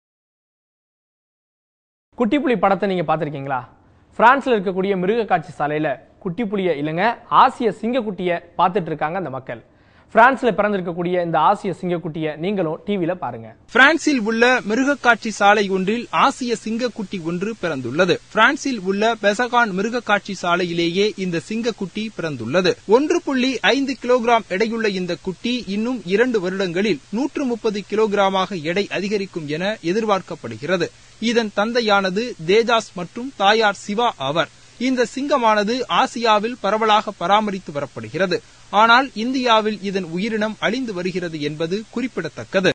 மக்கள் பிரான்ஸ்-ல பிறந்து இருக்கும் ஆசிய சிங்க குட்டி
குட்டிபுலி திரைப்படத்தை நீங்க பார்த்தீங்களா?
பிரான்சில் இருக்கிற  மிருக காட்சி சாலை-ல குட்டிபுலி-யை இல்ல ஆசிய சிங்க குட்டி-யை பார்த்துகிட்டு  இருக்காங்க மக்கள்  பிரான்ஸ்-ல பிறந்து இருக்கும் இந்த ஆசிய சிங்க குட்டி-யை நீங்களும் டிவி-ல பார்த்து கொஞ்சுங்க...